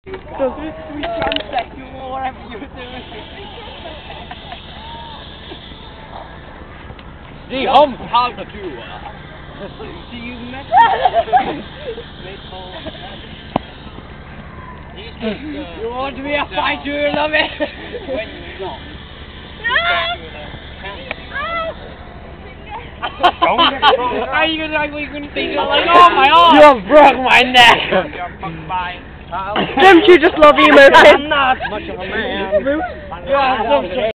Basically I got owned
This video makes me smile! Another 2008 memory of us in Calderstones Park, me trying to kick Ste's arse and getting my own kicked instead :(